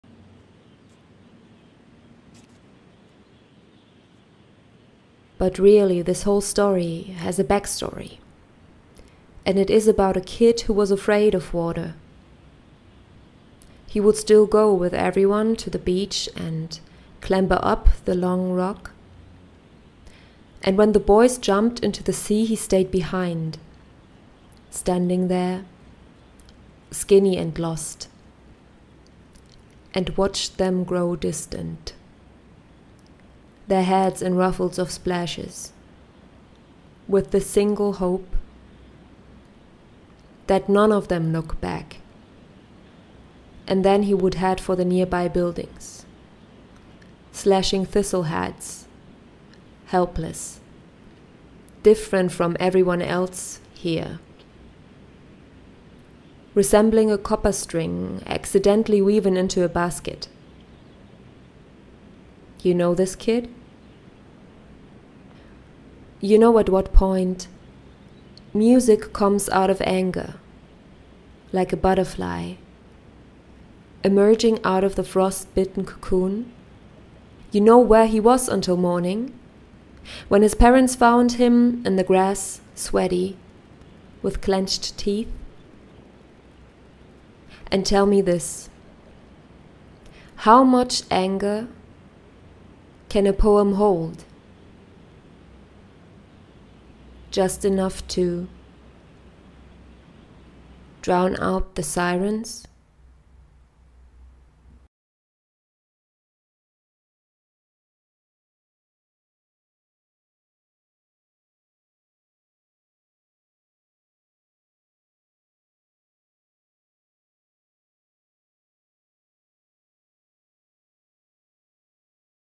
Ostap Slyvynsky (UKR) - Orpheus (performed by Marie Hahne (Taiwan))
ORPHEUS

But really this whole story

has a backstory, and it is about
a kid who was afraid of water.
But he would still go with everyone to the beach and clamber up
the long rock,
and when the boys jumped into the sea, he stayed behind,
standing there, skinny and lost,

and watched them grow distant,

their heads in ruffles of splashes,
with the single hope —
that none of them look back.
And then he would head for the nearby
buildings, slashing thistle heads, helpless,
different from everyone else here —

resembling a copper string, accidentally
weaved into a basket.
You know this kid?
You know at what point
music comes out of anger, like a butterfly
emerging out of a frostbitten cocoon?
You know where he was until morning, when
his parents found him in the grass, sweaty,

with clenched teeth?

And tell me this: how much anger can a poem hold?
Just enough to
drown out the sirens?

text: Ostap Slyvynsky (UKR)
translation: Anton Tenser and Tatiana Filimonova
performed by Marie Hahne (Taiwan)

a part of "Poems from Ukraine" project by ProEnglish Theatre from Ukraine